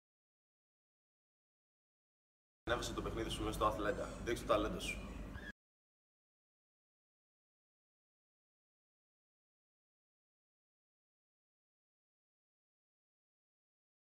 Dinos Mitoglou talks about Athlenda
A young and talented Greek PRO athlete, Dinos Mitoglou, knows best what you need to get promoted. Upload your videos to Athlenda and make a step forward towards your Basketball Career.